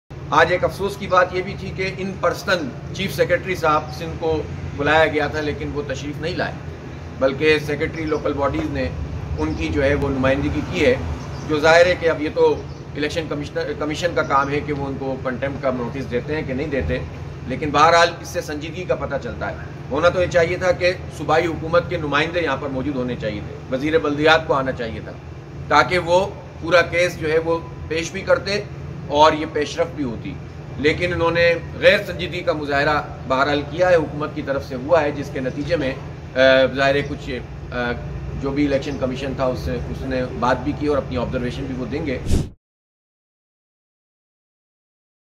الیکشن کمیشن کی سماعت میں صوبائی حکومت کے نمائندوں کا شریک نہ ہونا افسوسناک ہے۔ حافظ نعیم الرحمن
الیکشن کمیشن کی سماعت میں وزیر بلدیات اور صوبائی حکومت کے نمائندوں کا شریک نہ ہونا افسوسناک ہے۔ حافظ نعیم الرحمن